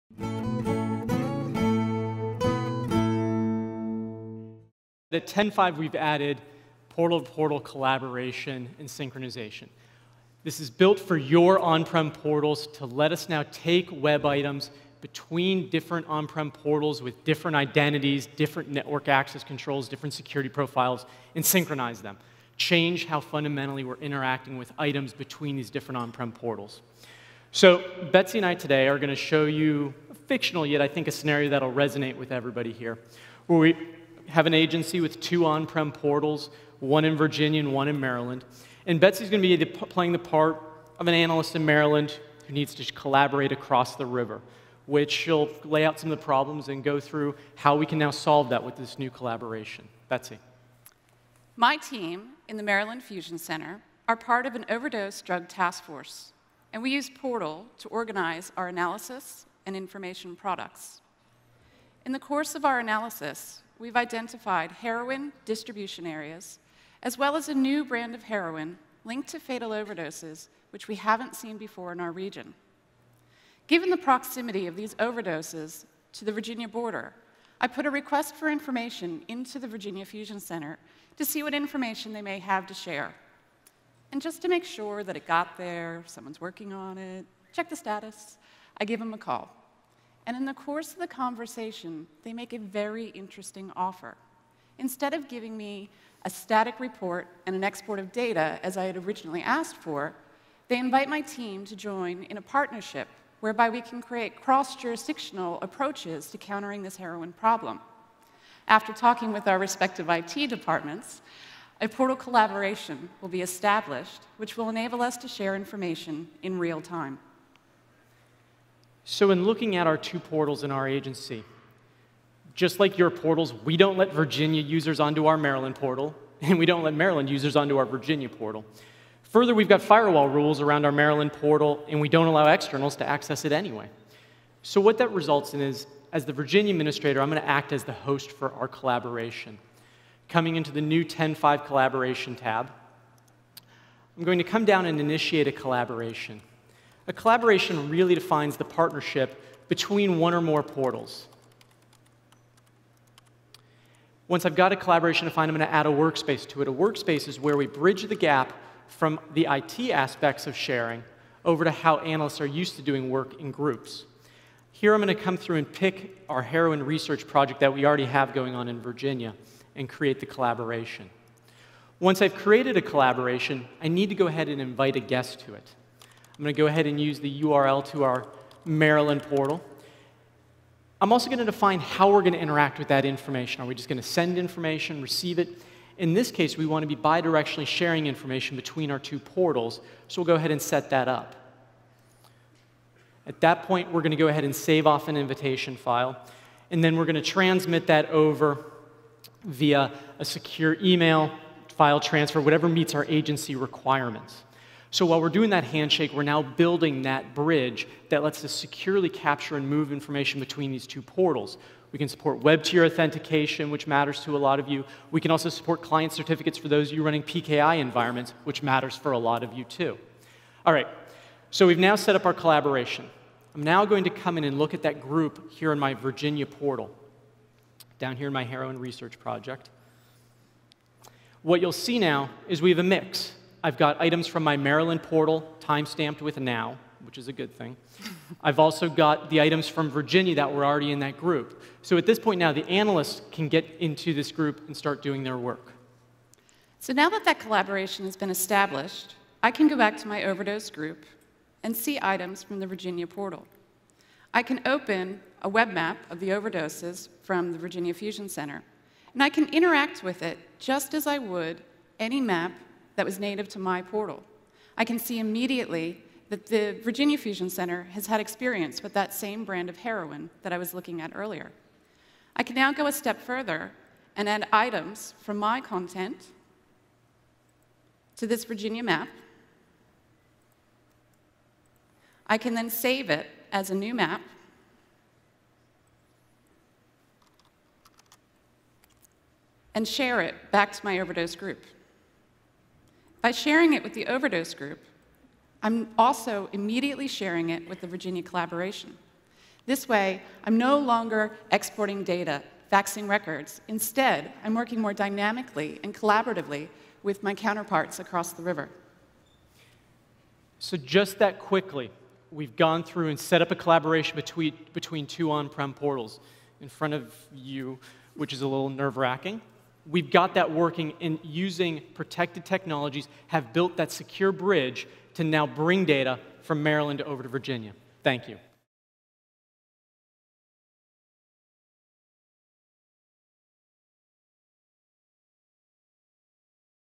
ArcGIS Enterprise Portal Collaboration
ArcGIS Enterprise provides a dramatic enhancement that supports portal-to-portal collaboration.